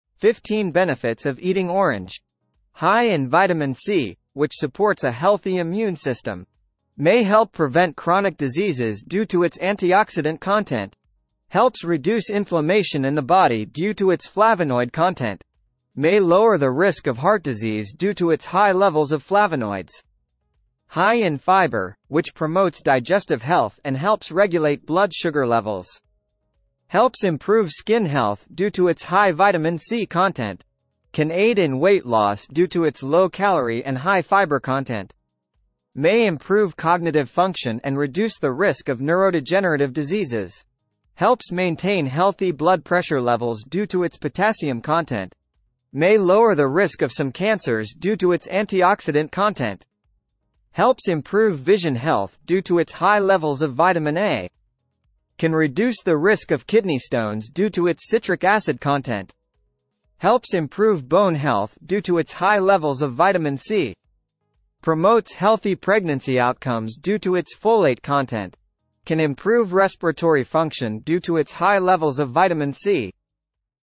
15 benefits of eating orange"
High in vitamin C, which supports a healthy immune system
May help prevent chronic diseases due to its antioxidant content
Helps reduce inflammation in the body due to its flavonoid content
May lower the risk of heart disease due to its high levels of flavonoids
High in fiber, which promotes digestive health and helps regulate blood sugar levels
Helps improve skin health due to its high vitamin C content
Can aid in weight loss due to its low calorie and high fiber content
May improve cognitive function and reduce the risk of neurodegenerative diseases
Helps maintain healthy blood pressure levels due to its potassium content
May lower the risk of some cancers due to its antioxidant content
Helps improve vision health due to its high levels of vitamin A
Can reduce the risk of kidney stones due to its citric acid content
Helps improve bone health due to its high levels of vitamin C
Promotes healthy pregnancy outcomes due to its folate content
Can improve respiratory function due to its high levels of vitamin C.